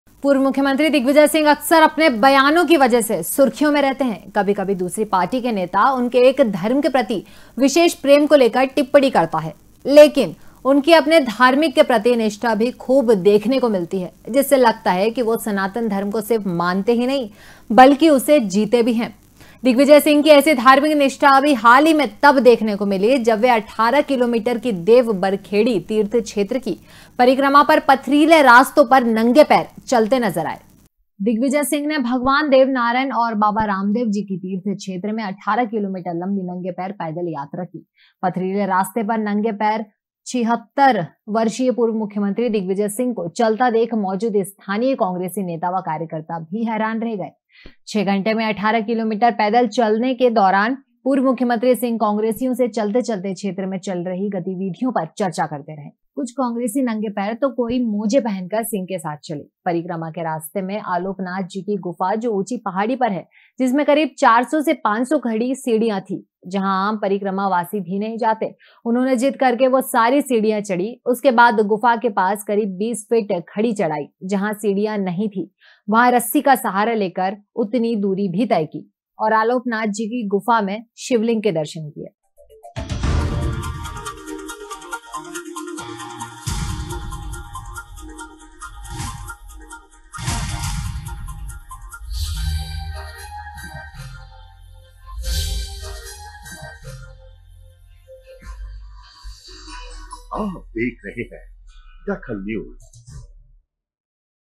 DAKHAL NEWS दिग्विजय सिंह ने 18 किलोमीटर की परिक्रमा की
दिग्विजय सिंह ने 18 किलोमीटर की परिक्रमा की
इस दौरान वह पथरीले रास्तों पर नंगे पैर चले